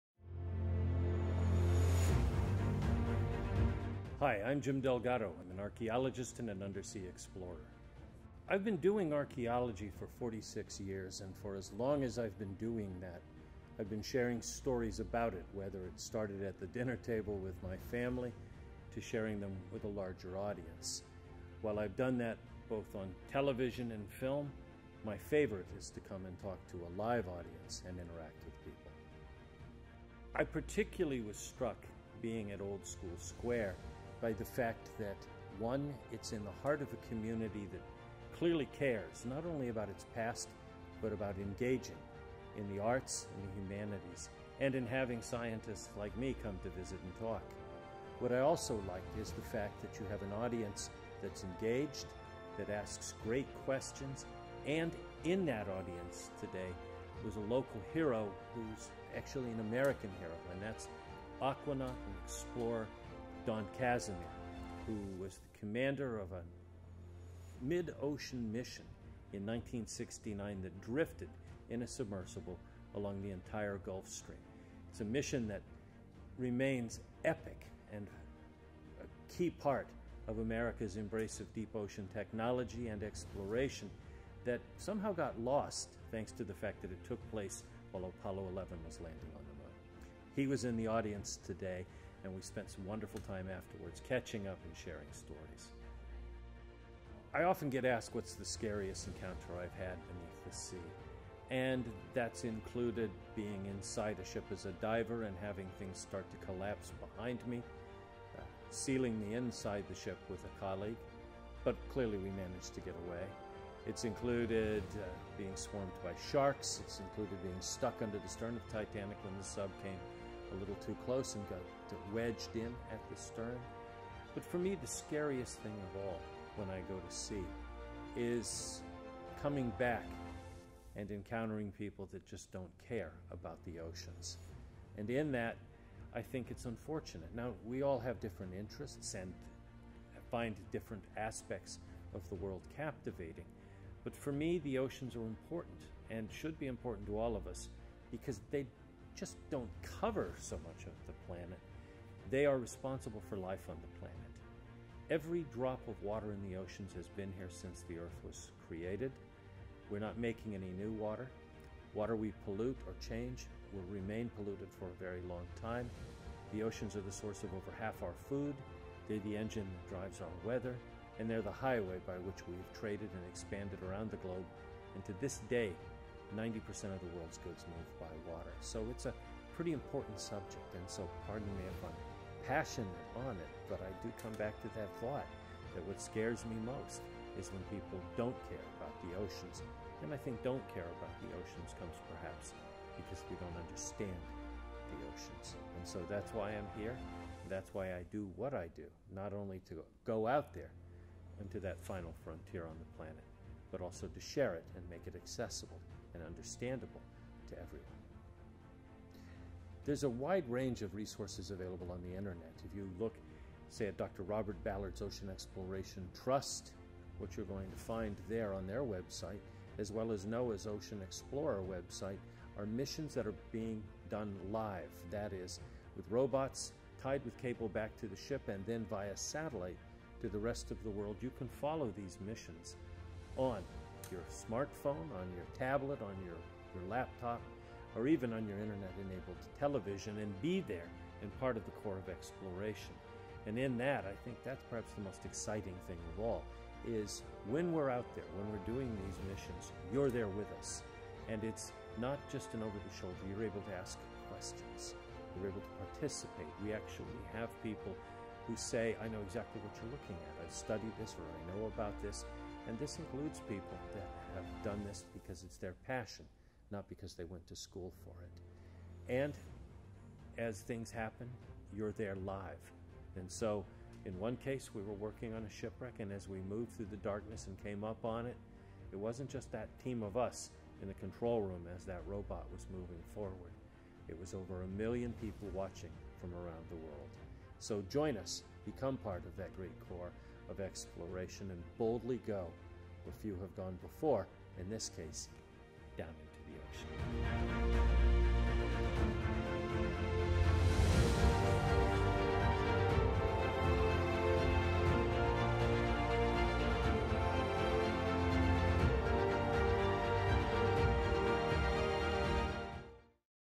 World Renowned Maritime Archaeologist James Delgado
Our most recent interview was with world renowned maritime archaeologist Dr. James Delgado during the Old School Square Crest Theatre RobertDCapin lecture series.

Dr. Delgado has spent nearly four decades in underwater exploration. A veteran of over 100 shipwreck investigations around the world, his work has included RMSTitanic, USSMonitor, USSArizona (BB-39), Sub Marine Explorer, the buried Gold Rush ships of San Francisco, and KhubilaiKhan's legendary lost fleet.

Filmed and edited by Sko Gudiño